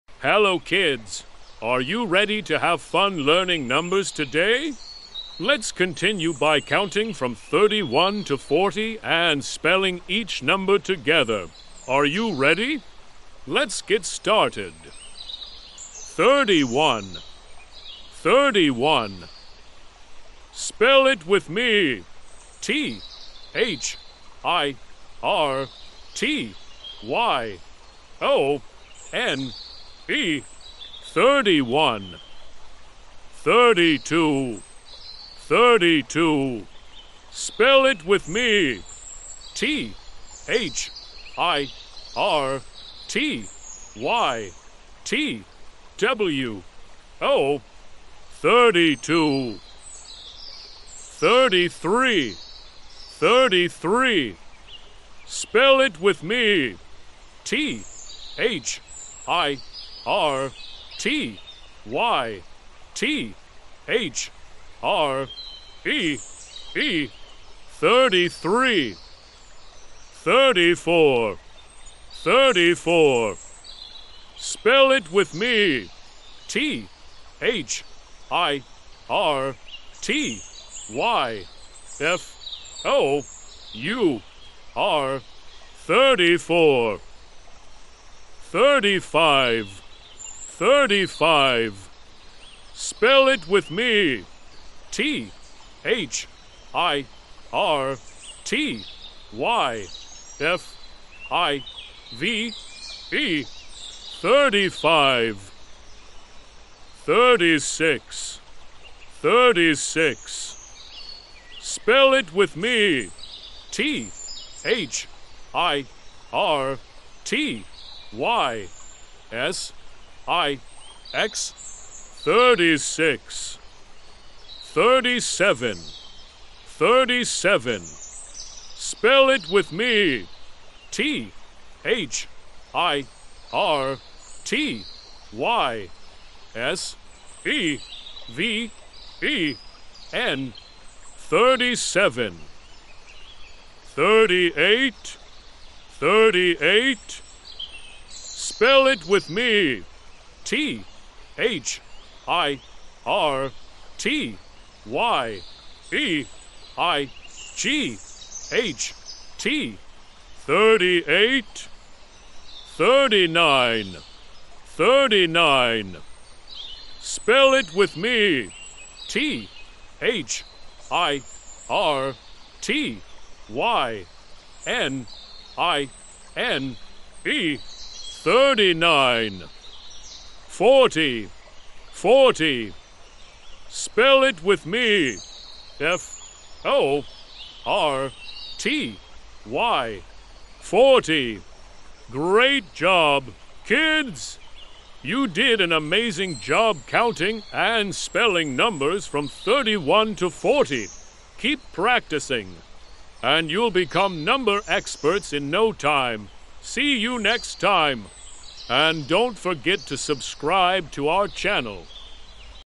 Learn Numbers: Counting from 31 to 40 with Spelling | Part 3 | #123
Join us for an exciting adventure of learning numbers! 🌟 In this engaging video, children will have a blast counting from 31 to 40 while also learning to spell each number. 🎉 With colourful visuals and cheerful narration, kids will enjoy every moment of this educational journey. 🎈 Let's dive into the world of numbers together and make learning fun! 📚🔢 Don't miss out on the excitement - hit play and let the counting begin! 🌈✨